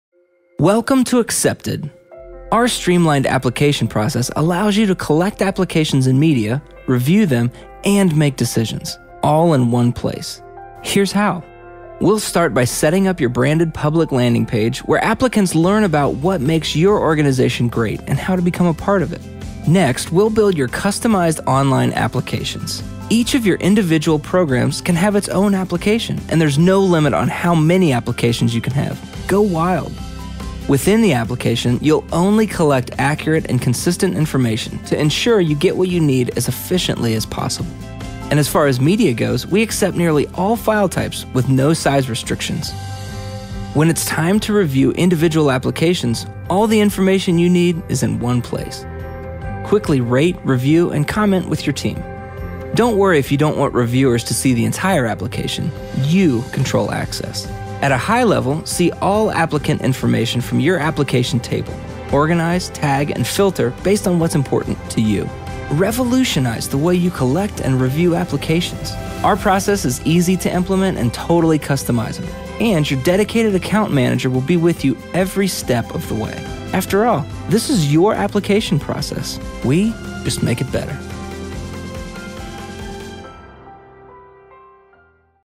Acceptd Customized Application
Create a simple, straightforward application process for your applicants. With a clean layout, multiple question types, and unlimited media uploads, you'll want to apply to your program yourself.